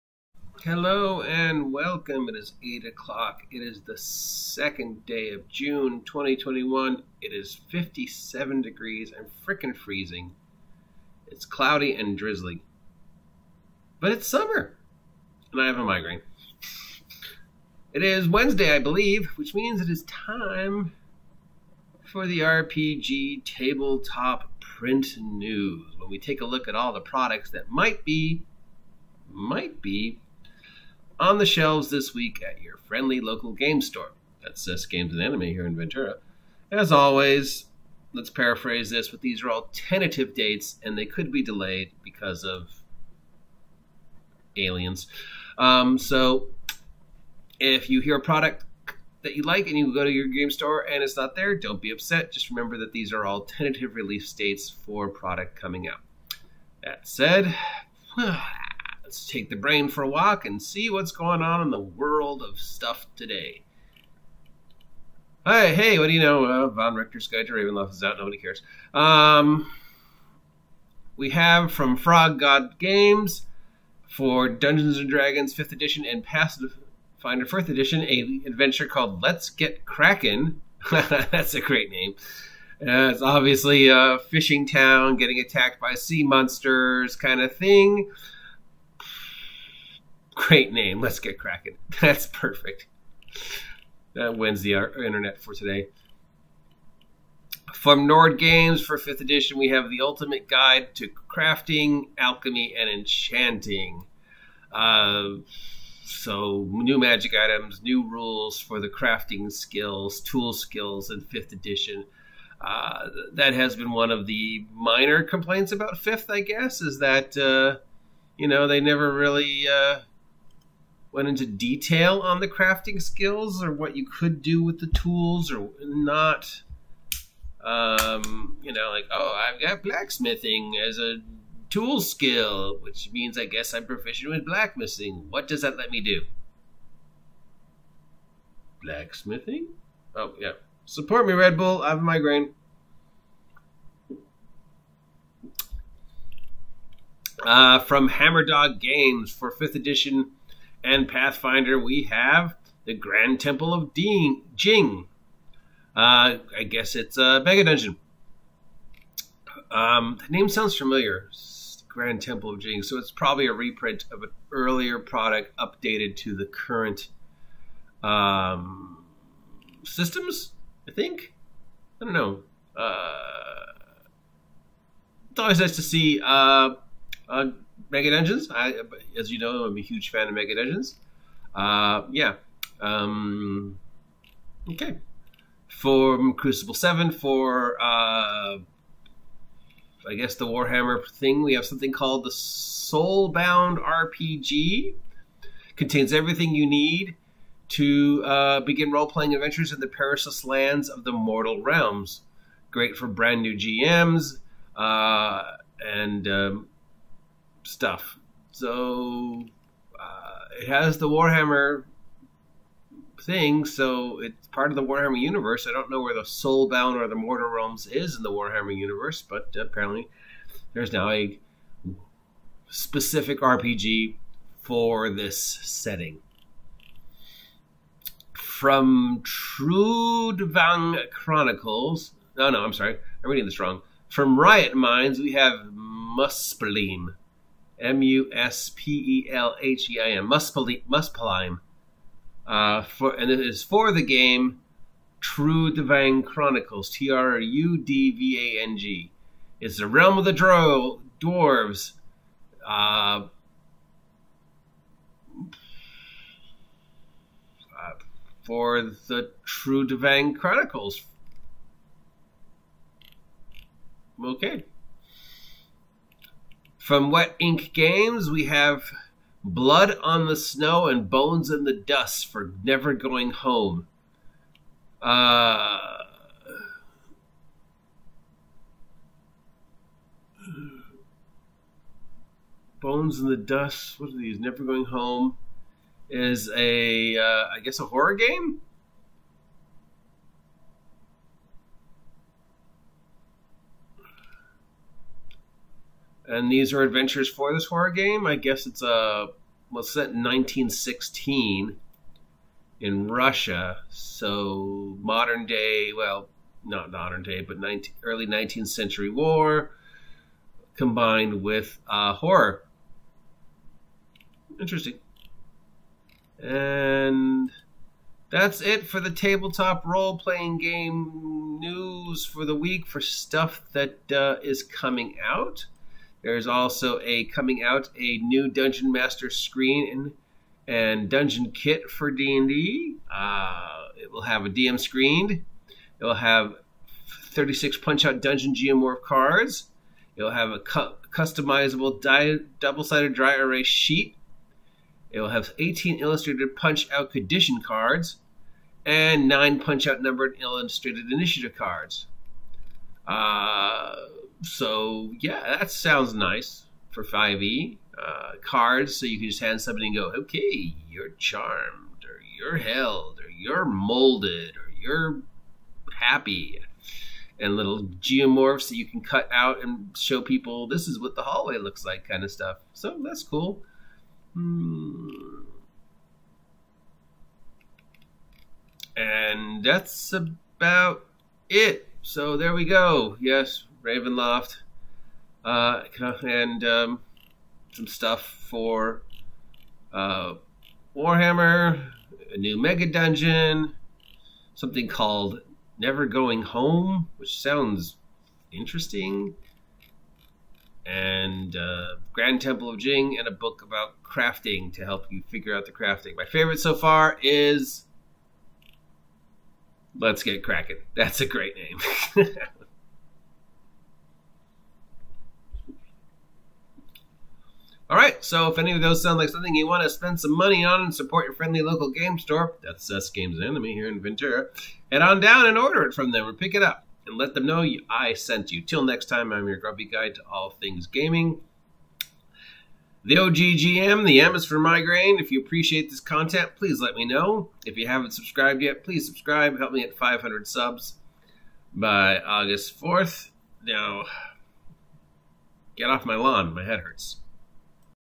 TTRPG Print news for the week. PG13
The following TTRPG products should be on the shelf at your FLGS this week.
Support the FLGS.